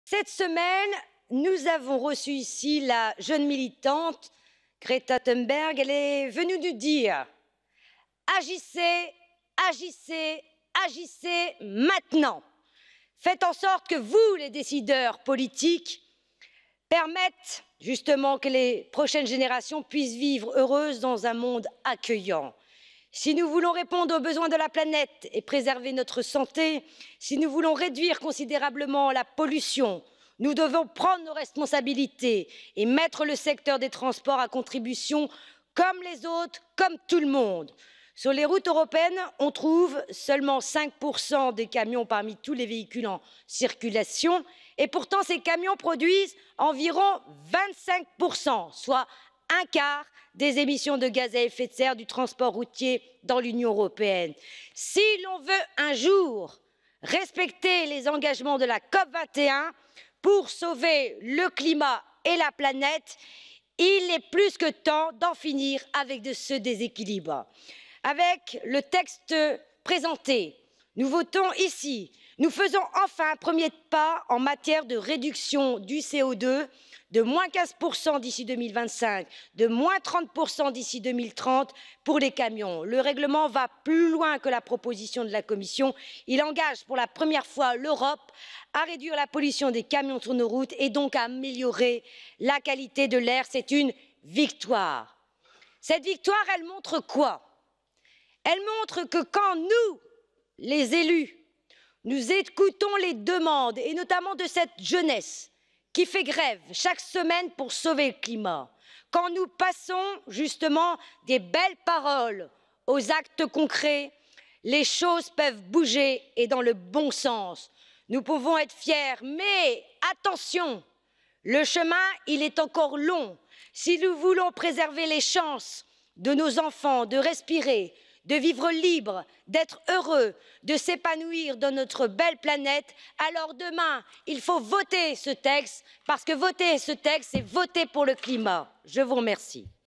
Pour la première fois l’Europe s'engage à réduire les émissions des camions
Intervention de Karima Delli en plénière du Parlement européen à l'occasion du débat sur les normes applicables aux émissions de CO2 des véhicules utilitaires lourds neufs.